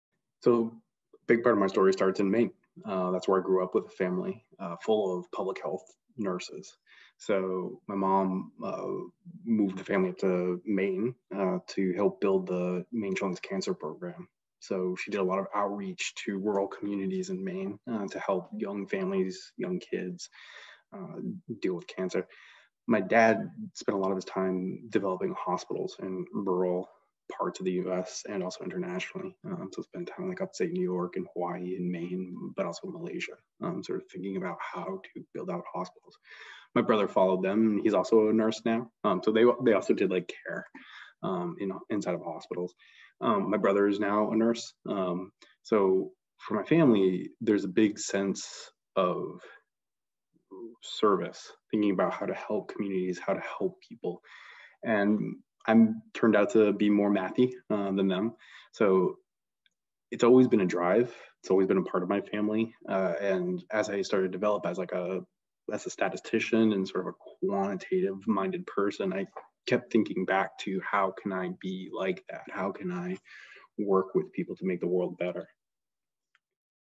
Origin Story, Mike Baiocchi E&PH Spotlight
This is video 1 of a 7 part playlist, capturing a video interview with Assistant Professor of Epidemiology & Population Health at Stanford University, Mike Baiocchi, that took place on April 28, 2021 for the E&PH Spotlight Interview series. In this clip, Dr. Baiocchi answers the following question(s): "Can you start by telling us a bit about yourself? How did you get here (to Stanford Epidemiology & Population Health)? Was there something in particular that attracted you to the fields of science, health and disease?"